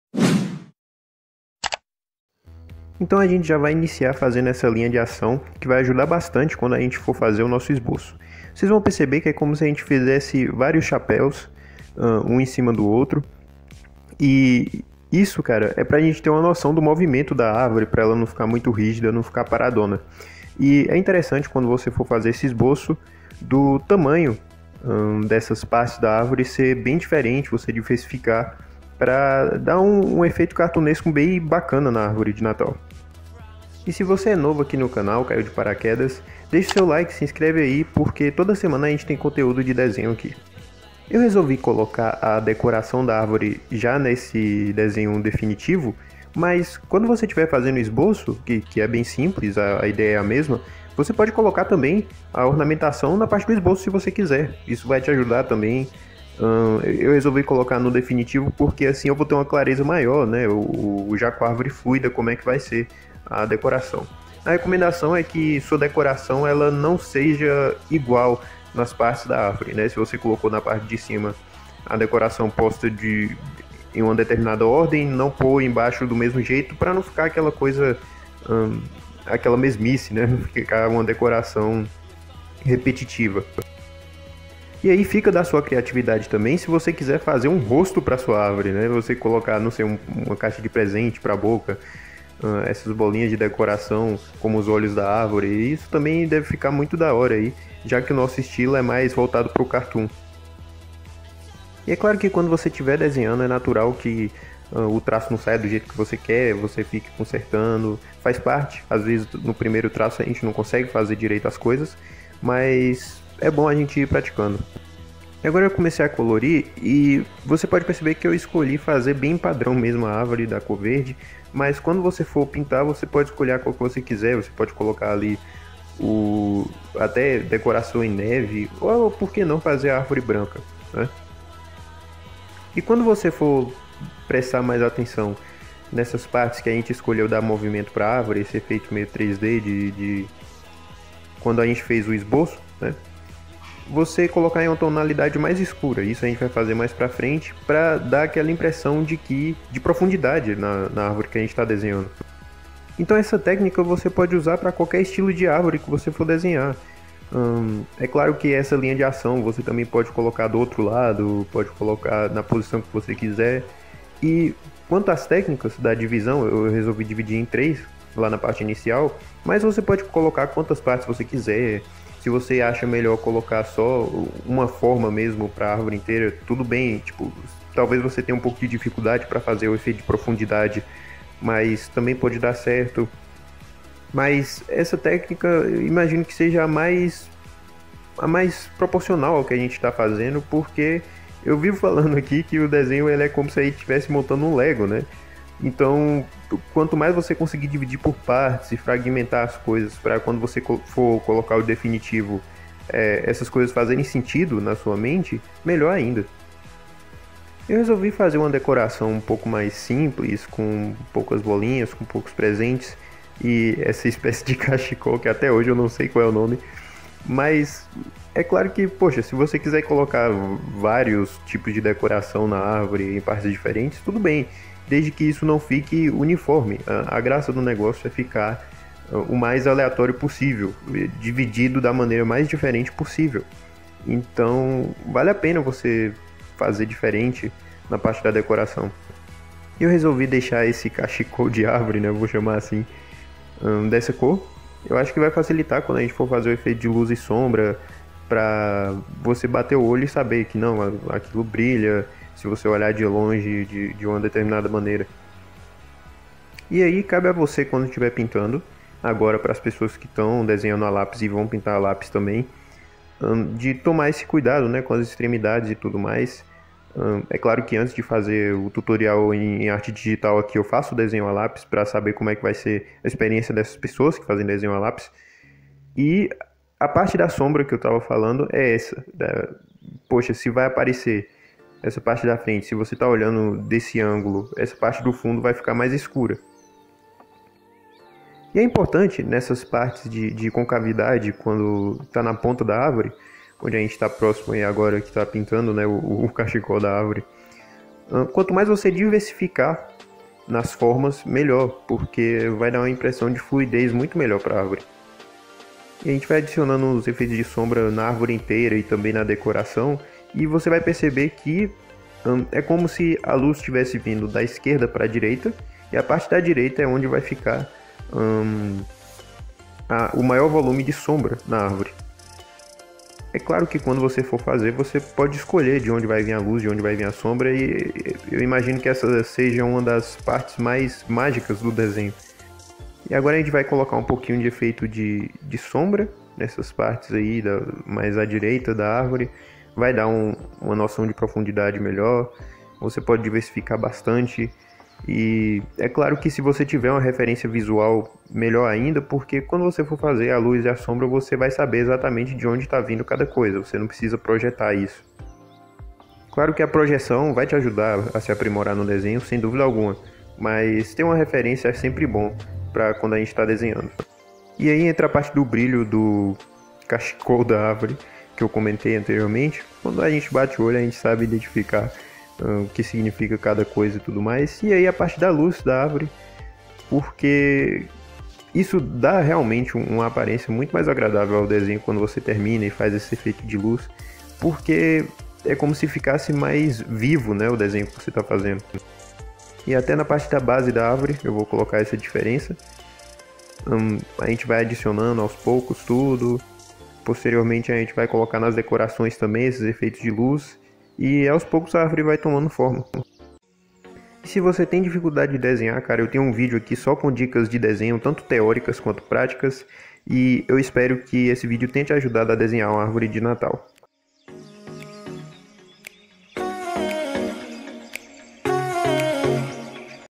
PASSO A PASSO: COMO DESENHAR UMA ÁRVORE DE NATAL
O Natal já está batendo em nossas portas e muitas pessoas gostariam de aprender a desenhar uma árvore de natal por diversos motivos! Seja para decorar a casa, para presentear alguém ou até mesmo porque quer aprender a desenhar e vê que desenhar uma árvore de natal é uma maneira efetiva de ter esse novo hábito.
Nesse vídeo, você aprenderá um passo a passo de como desenhar uma árvore de natal, mesmo que você não esteja acostumado a desenhar e seja sua primeira vez desenhando.
Esse tutorial de desenho também serve para as pessoas que já sabem desenhar, mas querem aprender uma forma de desenhar uma árvore de Natal.
Você vai saber como fazer o esboço da árvore de natal, como construir seu traço definitivo e como colorir sua árvore de natal de maneira que dê vida ao que você desenhou!